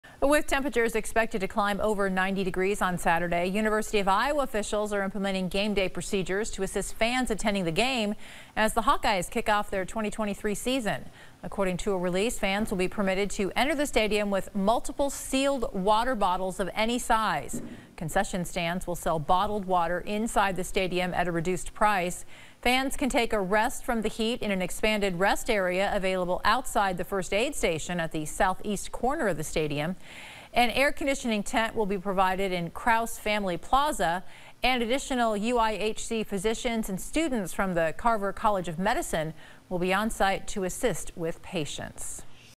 University of Iowa Officials Announce 'Beat the Heat' Gameday Plan
Temperatures are expected to climb over 90 degrees on Saturday for the Iowa Hawkeyes Football season-opener game on Saturday at Kinnick Stadium.